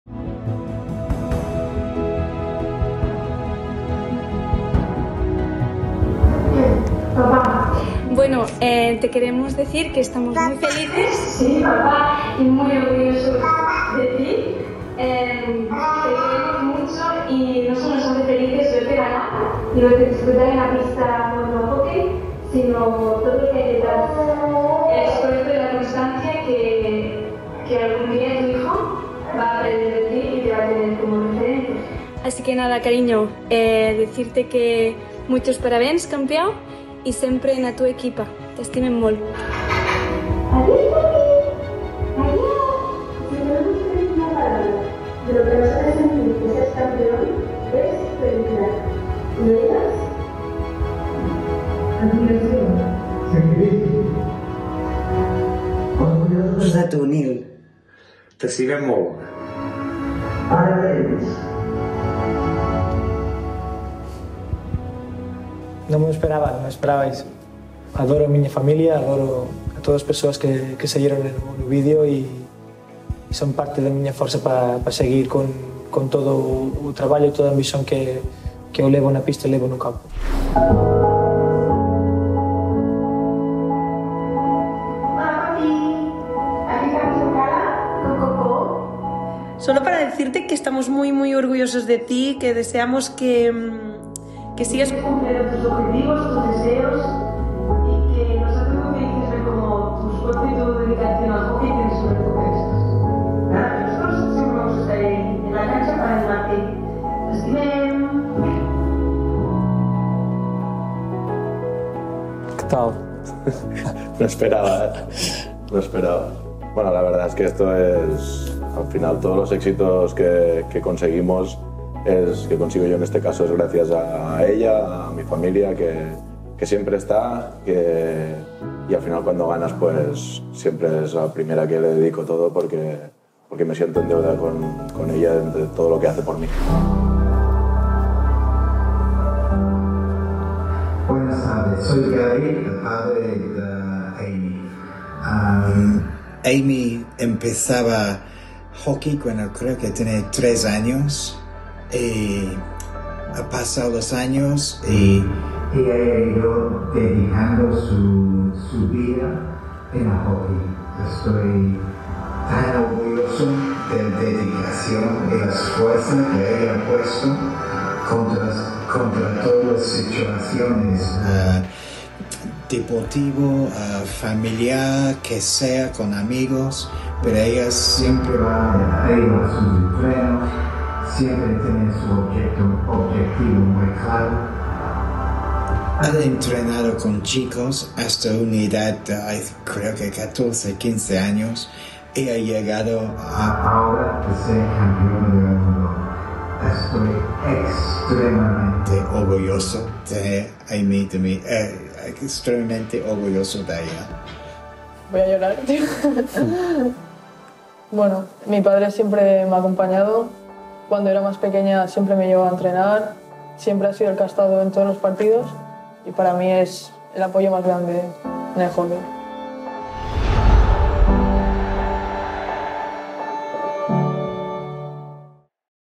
Hóquei em Patins | Homenagem aos Campeões do Mundo
Emoção na homenagem aos nossos campeões do Mundo de hóquei em patins pelas respetivas seleções, Nil Roca, Pau Bargalló e Aimée Blackman, que foram surpreendidos pelas famílias! ❤️